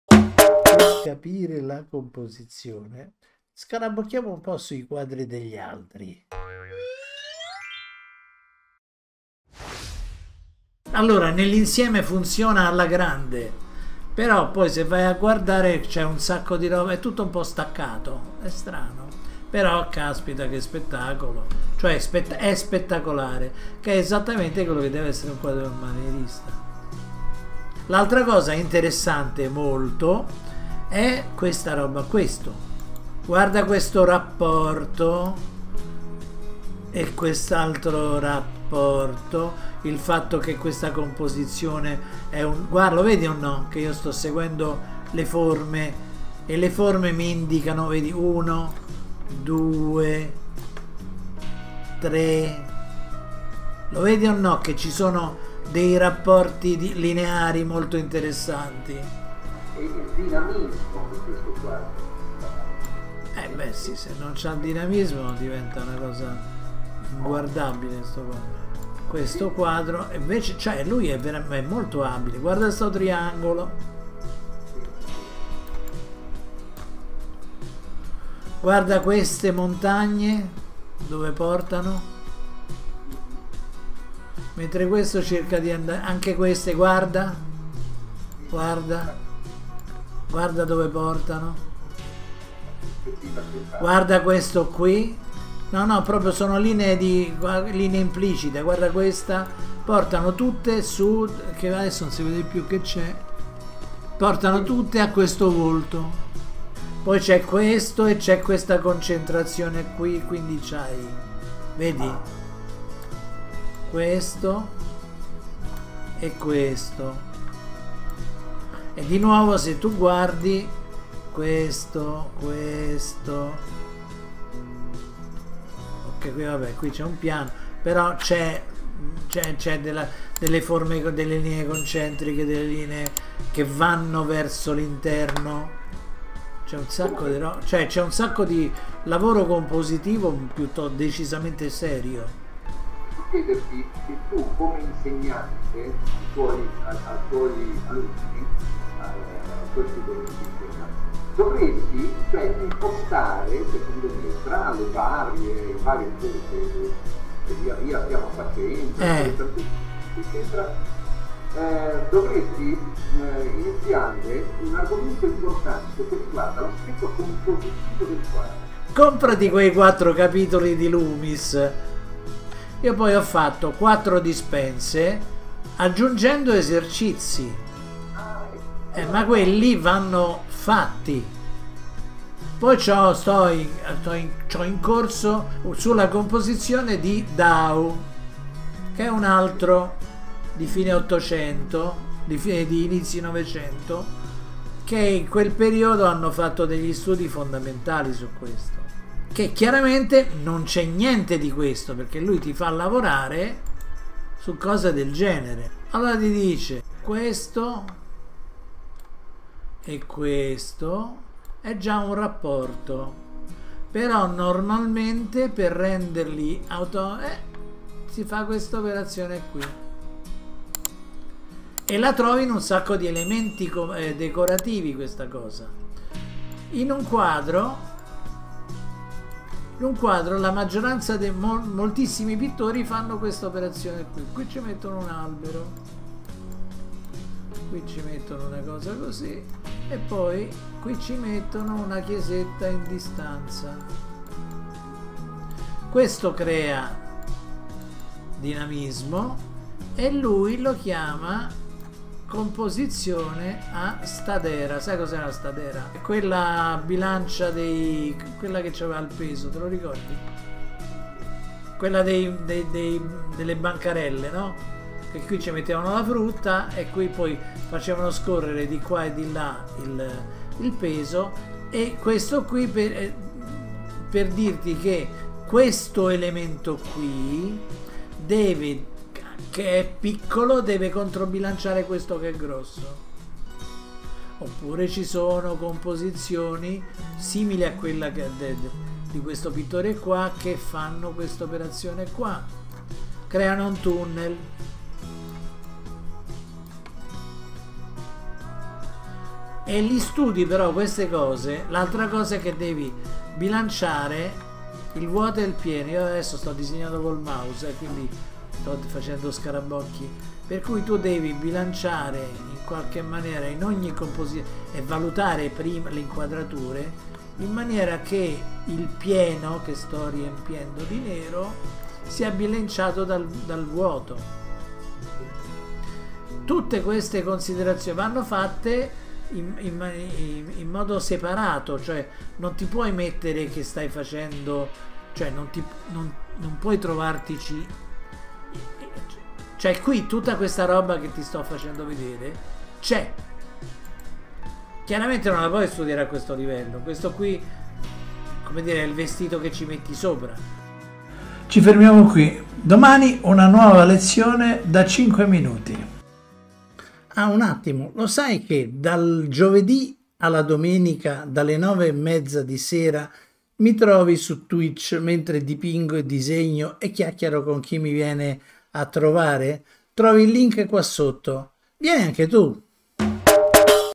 [1] CERCA LE LINEE IMPLICITE nei quardi dei grandi pittori!
Metti un trasparente sulla foto di un quadro famoso e cerca le linee che connettono e dirigono lo sguardo. Scoprirai molti trucchi di composizione che non ti aspettavi.

* Fai gli esercizi e (se vuoi) ENTRA NEL FORUM per una correzione

composizione in pittura#studio linee implicite#arte#